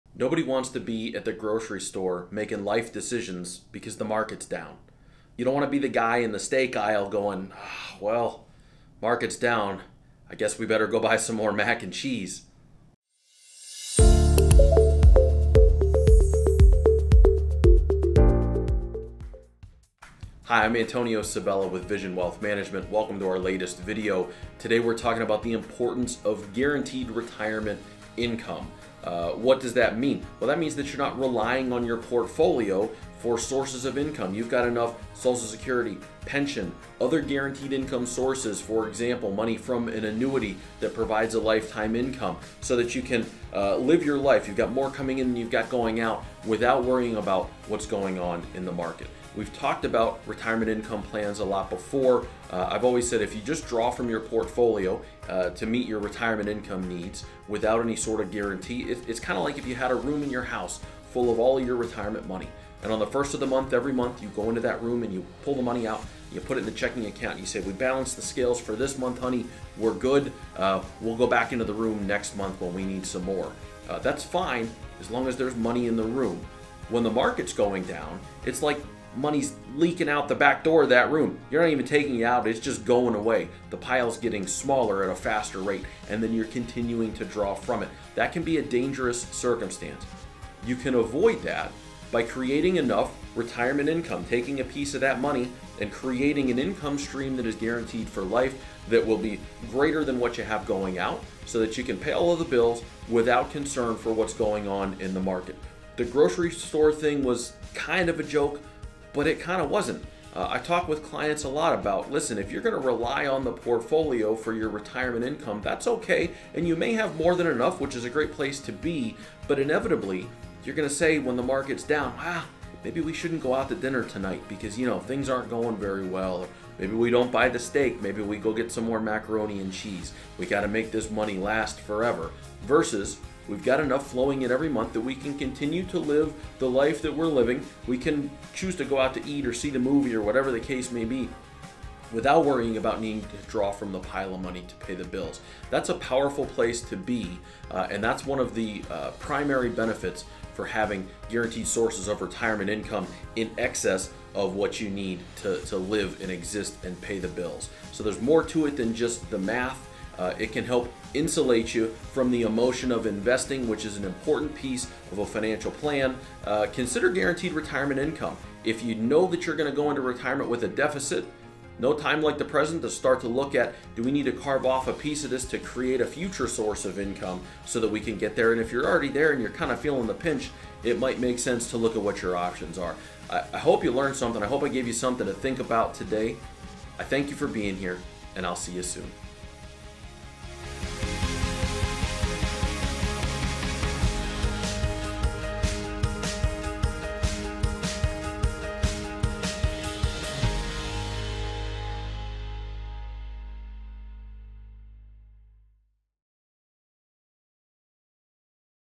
Why Guaranteed Retirement Income Matters
No one wants to run out of money in retirement. The key is understanding your income and expenses, and then maximizing your guaranteed sources of retirement income.